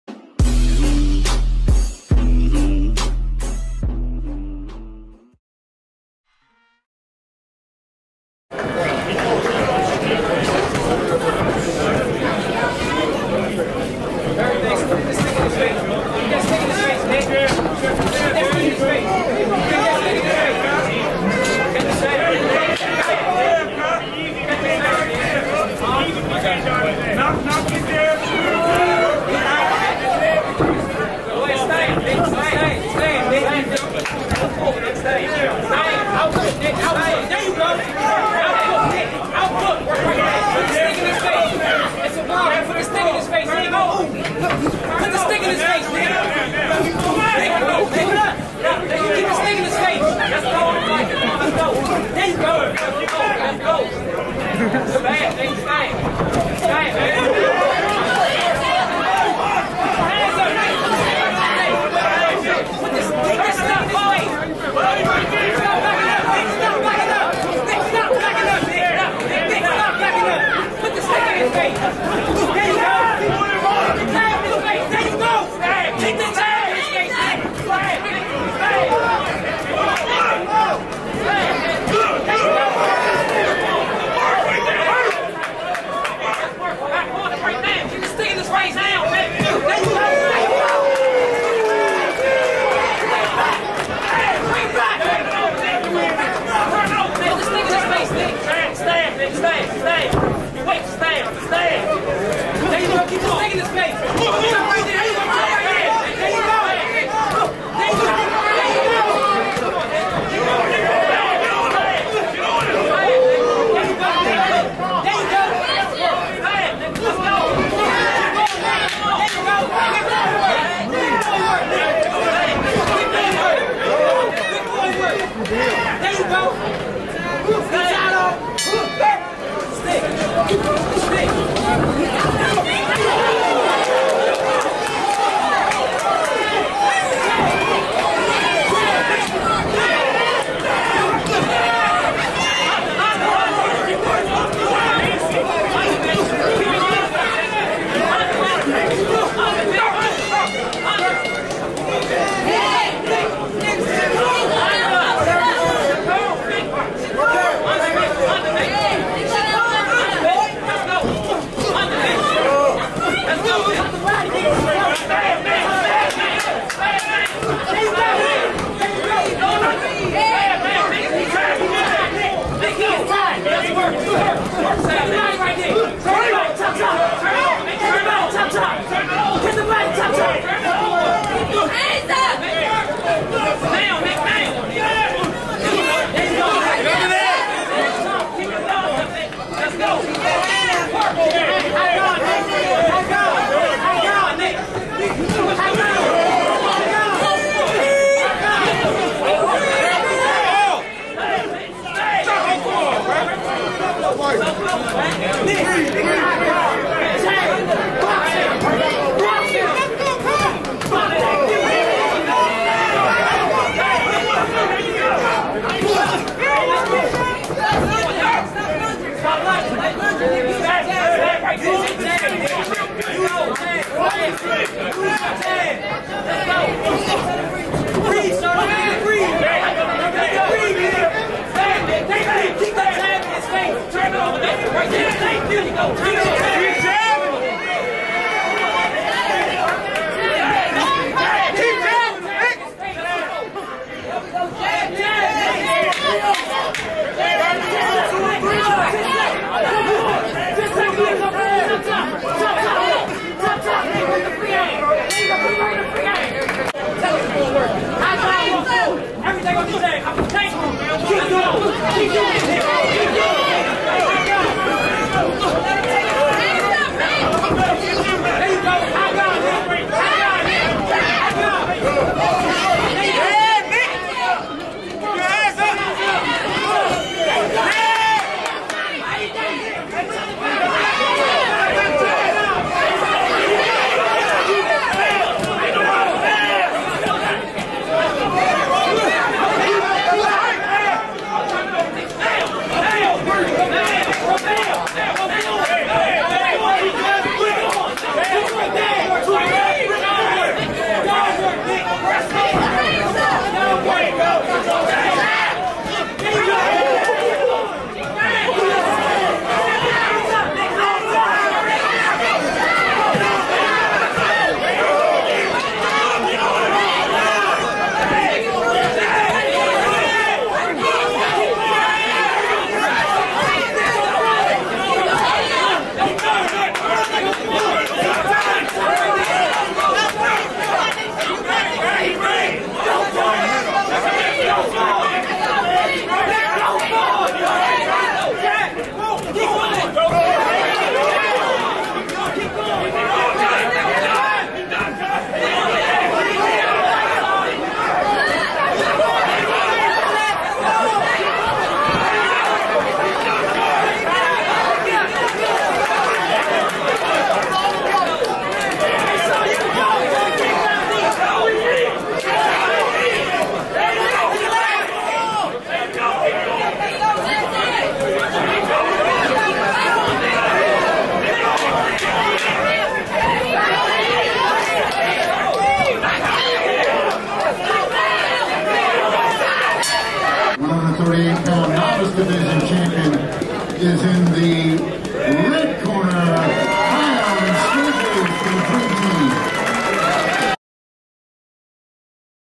Washington Golden Gloves | The Top1ck
2021 Washington DC, Golden Gloves. [Kiayon Strowbridge vs Nicholas Isacc ], 138
lbs division.